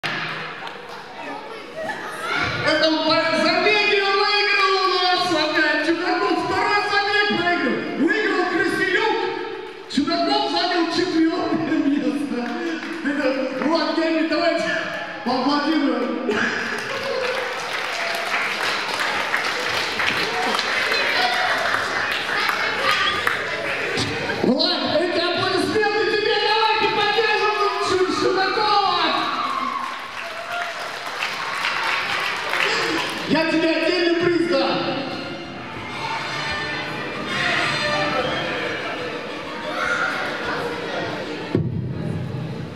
Расстройство Чудакова Влада. Турнир по боксу. СПК "ЯРОПОЛК", 5 ноября 2017 г.
5 ноября 2017 г. в СПК "Ярополк" состоялся внутренний турнир по самбо среди спортсменов клуба, посвящённый празднику- "День Народного Единства".
В соревновании приняли участие 50 воспитанников СПК "Ярополк" от 5 до 9 лет.